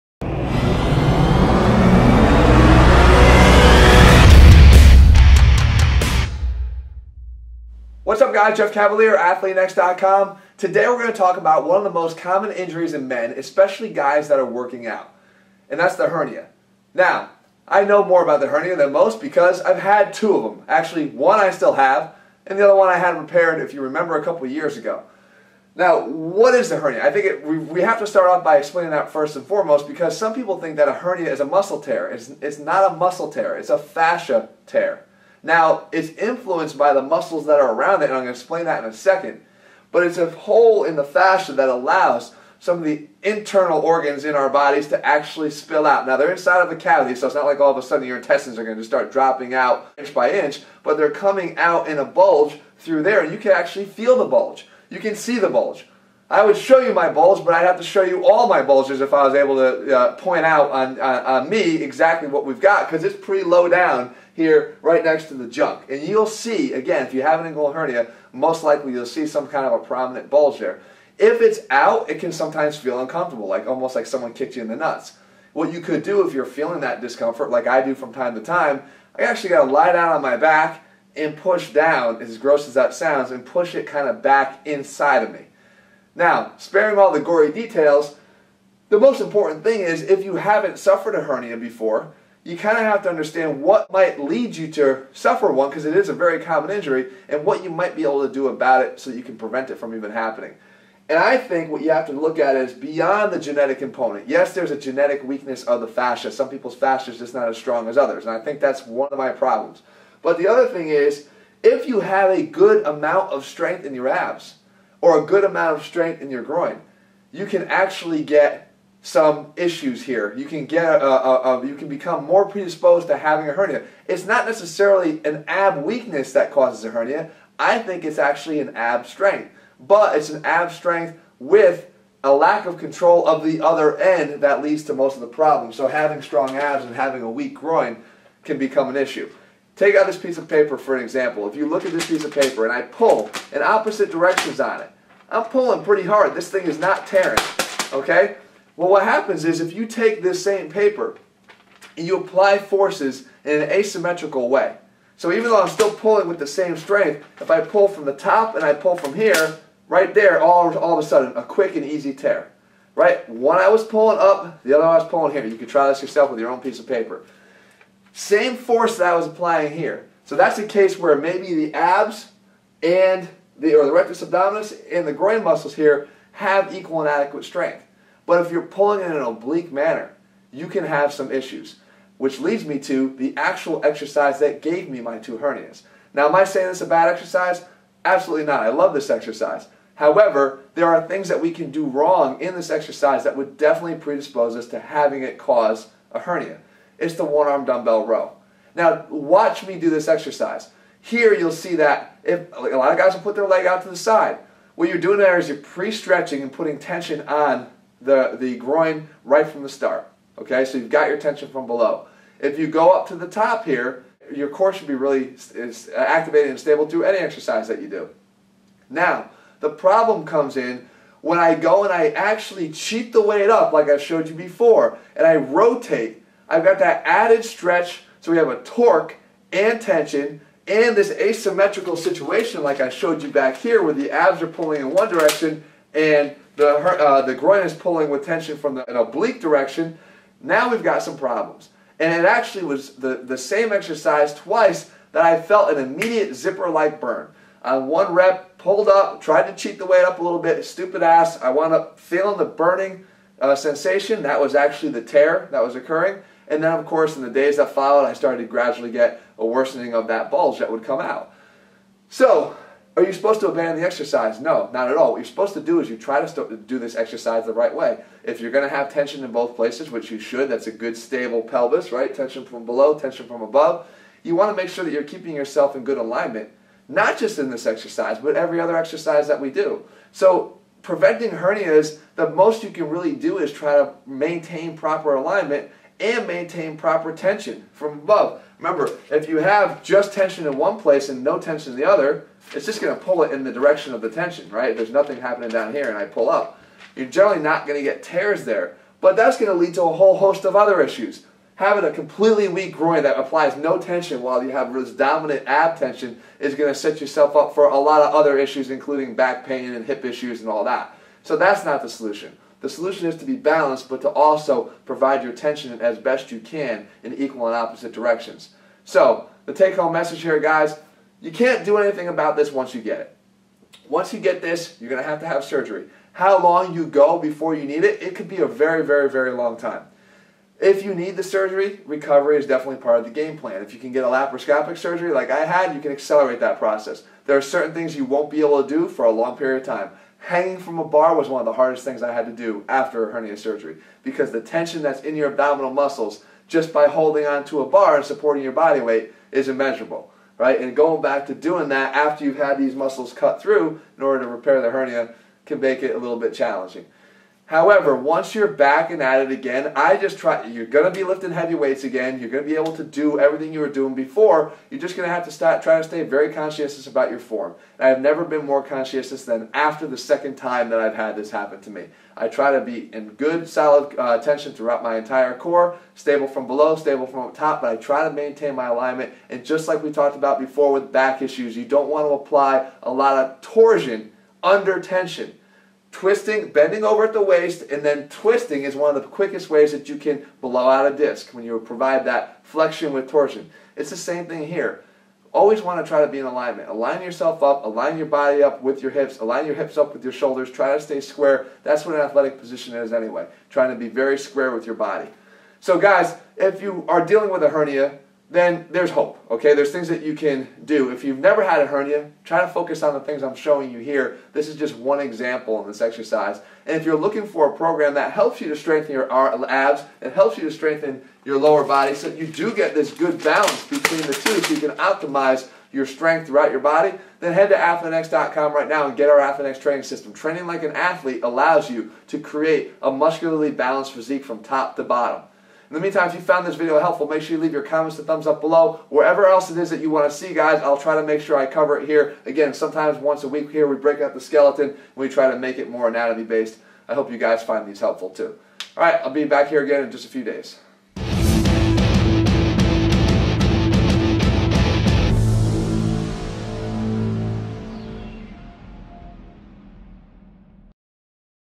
Hernias from Working Out (COMPLETE GUIDE!)
Hernias and inguinal hernia specifically, are one of the most common injuries men suffer from working out.  What causes them and what can you do about them?  In this video, I show you not only the exercise that caused both of my hernias but also what you can do to make sure you perform it correctly and prevent it from happening to you.

Inguinal hernias are the most popular type of hernia.  They are not tears in muscle but rather in fascia.  Some people are genetically born with weaker fascia than others.  This leaves them more susceptible to not only hernia injuries but also lower back strains as well.  The important point to consider however is that while muscles may not be torn in this injury, the muscles above and below the inguinal canal can influence your chance of injury.

The inguinal canal is bordered by the rectus abdominus at the top and the adductors and groin muscles from below.  The tension created by these two muscle groups across the fascia can lead to torsion and strain that causes a tear, when the force they generate becomes nonlinear.  As demonstrated in the video, when you pull on a piece of paper from both sides with equal force, you do not tear the paper.  When you change the direction of the force so that one is set at an angle that isn’t parallel to the other, you can create torque and tears.

I apply this to the one armed row exercise.  This back exercise is one of my absolute favorites for building a bigger back.  That said, the position of the movement can set you up for failure if you don’t remain conscientious about your core.  If you keep your core and back leg lined up you will perform this correctly.  That said, if in an attempt to lift more weight you cheat the dumbbell up by twisting your torso, you now apply torque to your fascia by rotating the abs in relation to the adductors.

This is just one example.  There are many others of course.  The key is that you need to maintain proper alignment as much as possible in your workouts and be sure to use good form.  You can train like an athlete and build proper muscle balance from head to toe by heading and starting the ATHLEAN-X Training System.